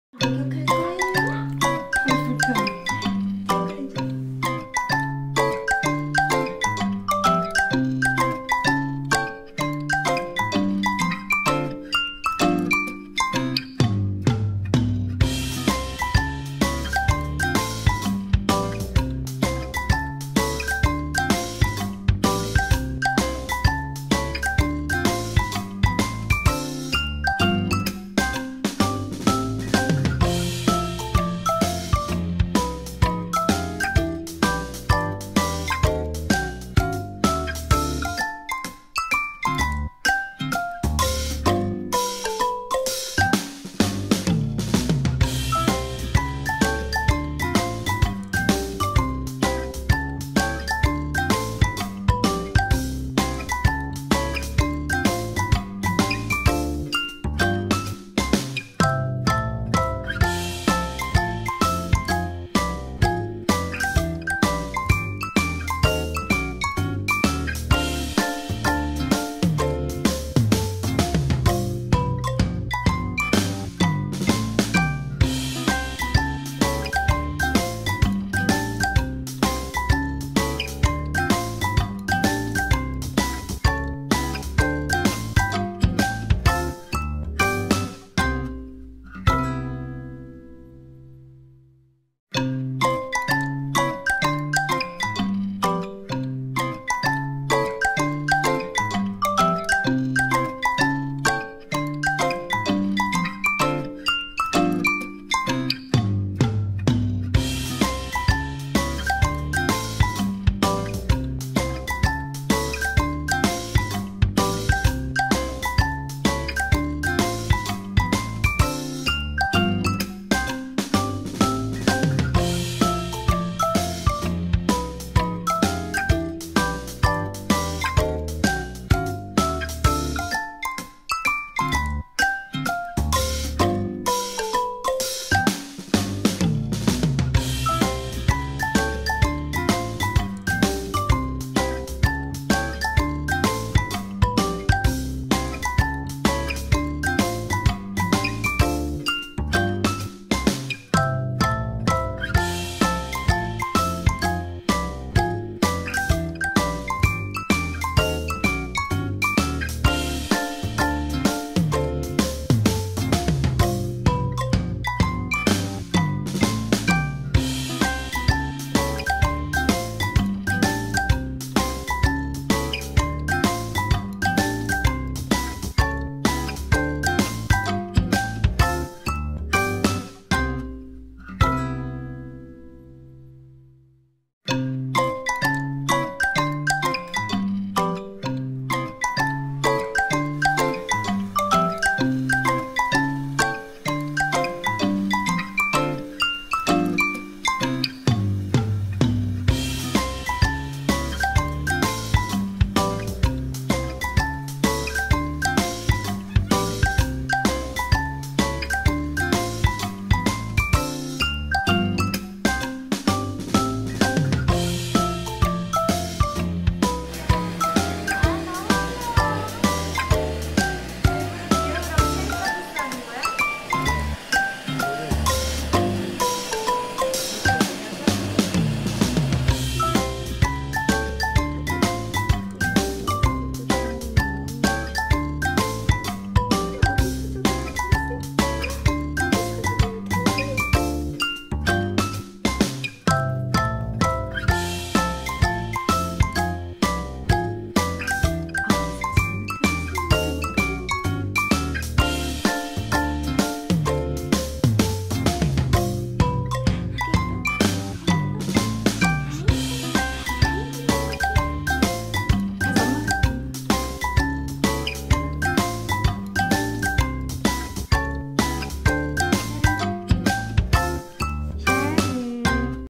쇼독 강아지 아기 포메라니안 목욕,드라이,빗질 꿀팁 대공개(feat 아기사자가 됐어요
상위 0.1% 쇼독 아기강아지 포메라니안 목욕,드라이,빗질 꿀팁들 알려드릴께요^^
우리나라 최고의 화이트 포메라니안 쇼독 견사 린치텐에서 전수받은 비법이에요
린치텐에선 아기 포메라니안 한마리에 천만원을 호가해요
그만큼 아이들이 족보도 좋구 건강하구 완벽한 포메라니안이에요
세계챔피언 하는 화이트 포메라니안은 거의다 린치텐 출신이에요
포메라니안은 이중모라 목욕,드라이,빗질을 잘 시켜야 털이 예쁘게 관리돼요
영상보구 참고하셔요^^
포메라니안 눈물자국도 목욕하면서 잘 없애주셔야하구요
6개월 된 귀여운 아기강아지 린치텐 출신의 화이트 포메라니안 샤를의 목욕하는 모습 심장어택이에요^^
목욕하구 뽀송뽀송하게 말리니까 아기사자가 됐어요ㅋㅋ
귀엽게 봐주세요ㅎ